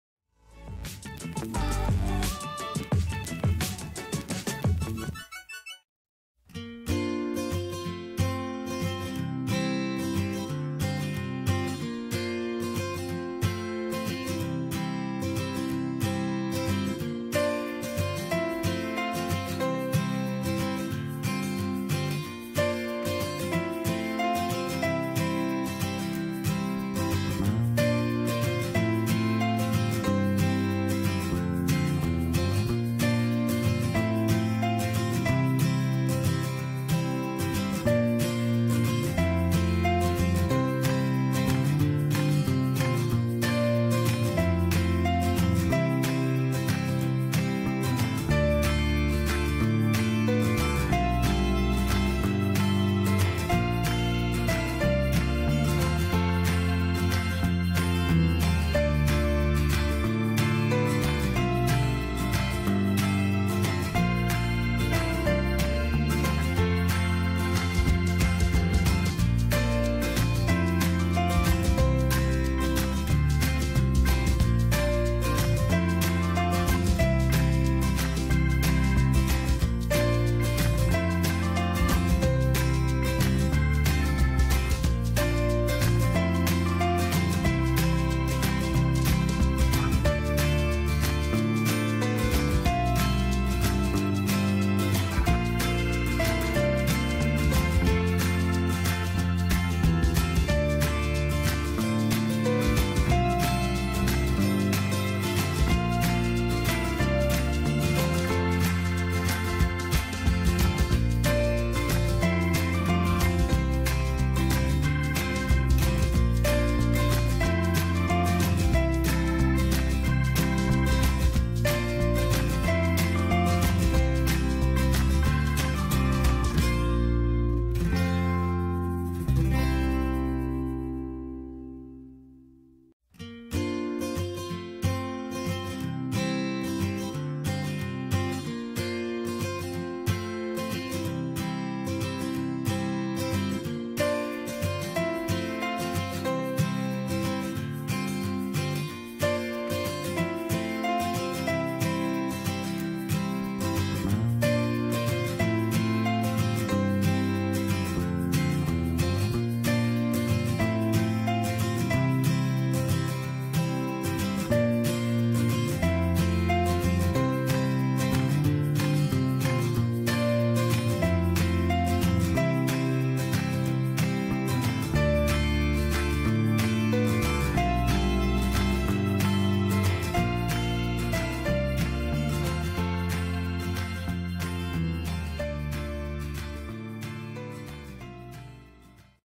How To Propagation Coleus Or Iler Flower
In this short video i will show you how to Propagation Coleus/Iler Flower.